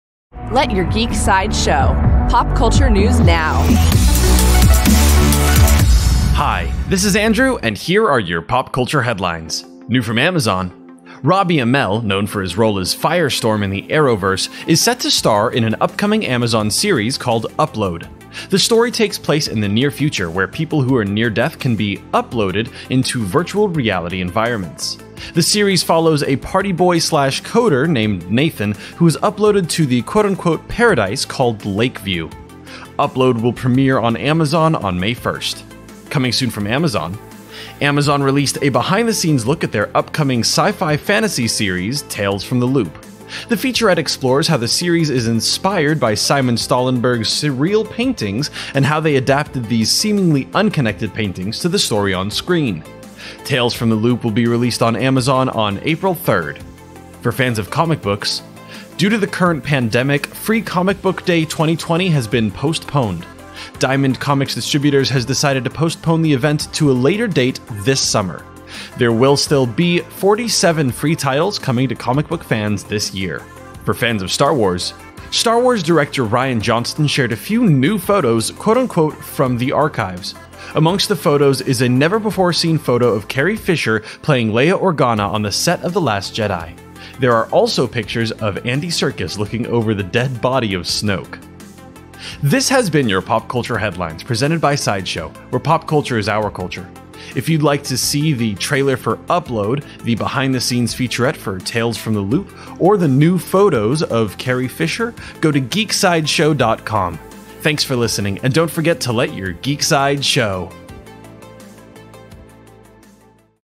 New Photos of Carrie Fisher - March 19th, 2020
New Photos of Carrie Fisher, Free Comic Book Day 2020 Postponed, New Tales from the Loop Featurette, New Trailer for Upload.